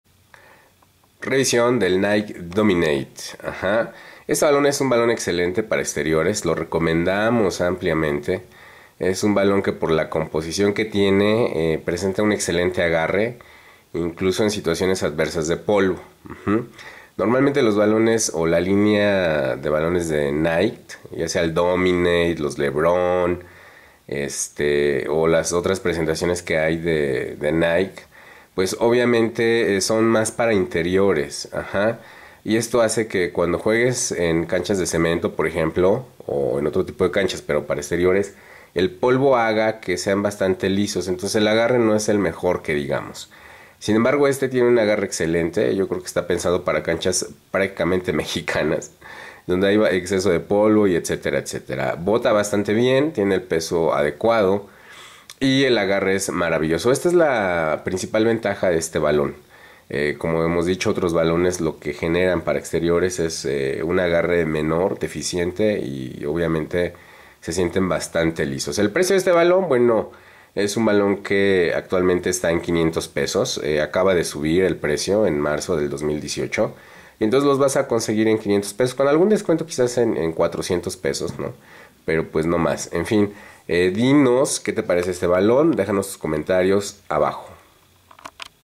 Nike Dominate
Review balón Nike Dominate, ampliamente recomendable para exteriores agarre insuperable